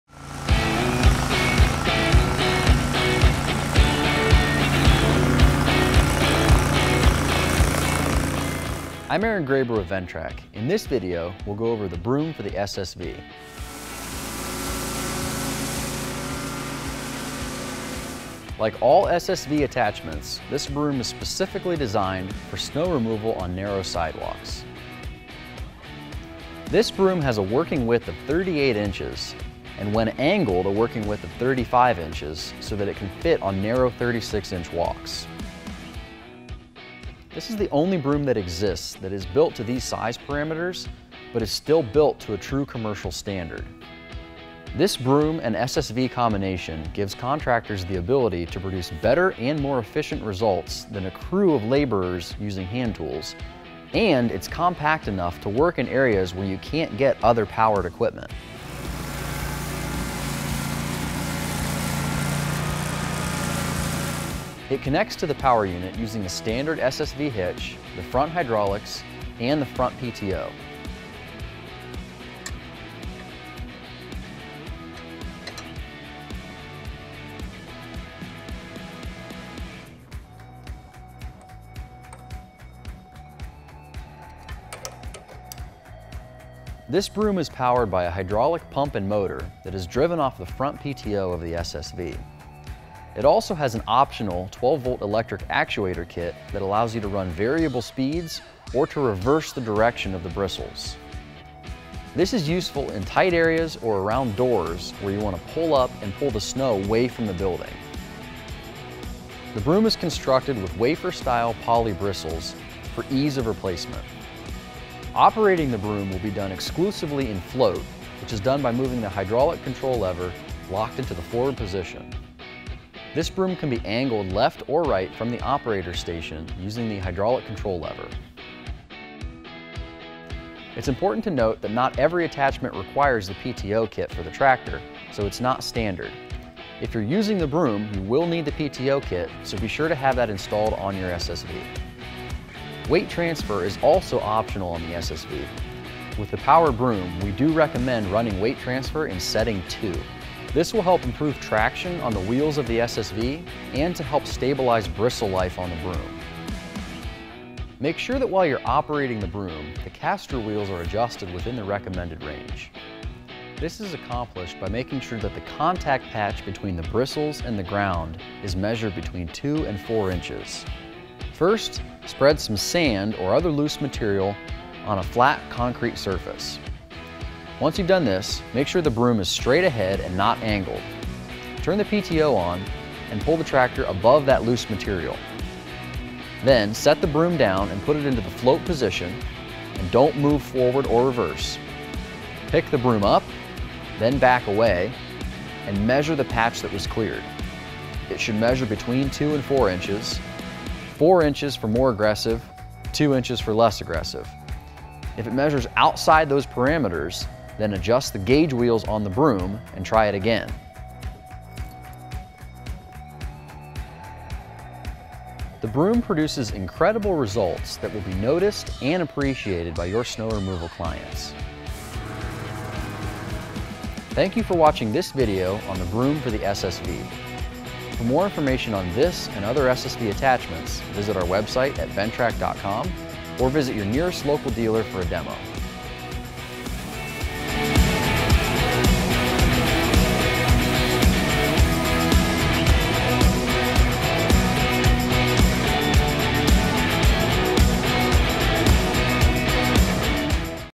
Quickly Remove Snow From Sidewalks With The Ventrac SSV Broom – Simple Start
When miles and miles of sidewalks get covered in snow this winter, there is a way to improve your efficiency and results with Ventrac’s Sidewalk Snow Vehicle.
The SSV Broom can outperform and replace a crew of hand laborers and hand tools with one sweep.
With results that will have your snow removal clients pleased, you can operate this compact vehicle where most other powered equipment cannot go.
The SSV Broom is designed specifically for narrow sidewalks with a working width of 38 inches and can be angled to 35 inches wide for standard sidewalk width. The only snow broom built with these size parameters that is still true to commercial standard.
Customize the aggressiveness of the brush action by adjusting the castor wheels depending on snow fall amounts and type of snow.
Clear doorways and approaches with a single sweep by simply changing the direction of the bristles to pull snow away from buildings.
The SSV Broom is truly a great addition to any snow removal team.

Products featured in this video:
Ventrac SSV & Broom: